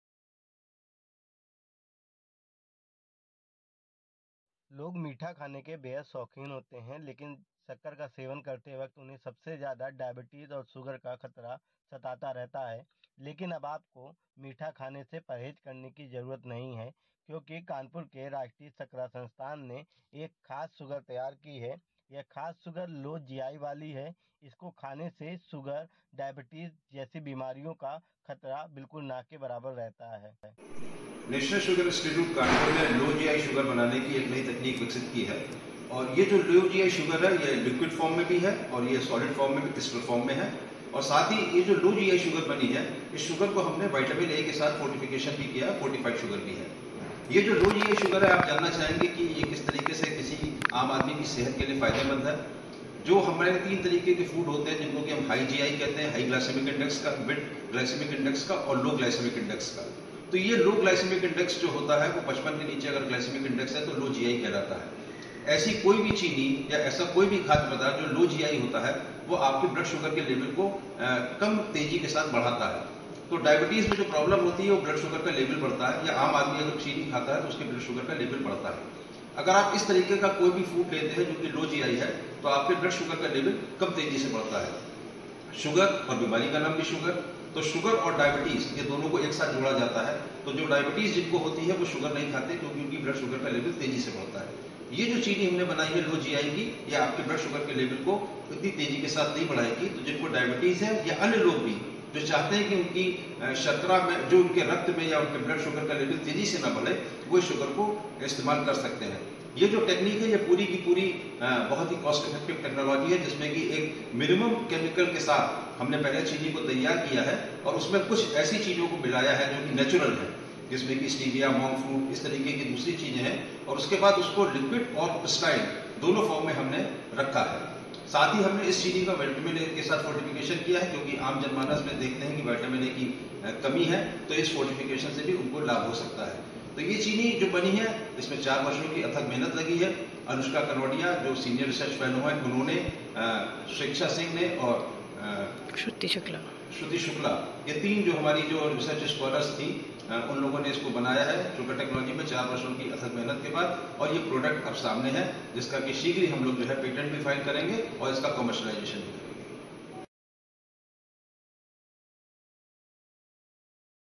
Health News : Diabetes की छोड़िए चिंता चाव से खाइए मीठा!
इस तकनीक की खासियत यह है कि इस तैयार शुगर से डायबिटीज का खतरा नहीं रहेगा बल्कि लोगों में विटामिन ए की कमी को भी यह शुगर पूरी करेगी.
The specialty of this technology is that there will be no risk of diabetes from this prepared sugar, but this sugar will also fulfill the deficiency of Vitamin A in people.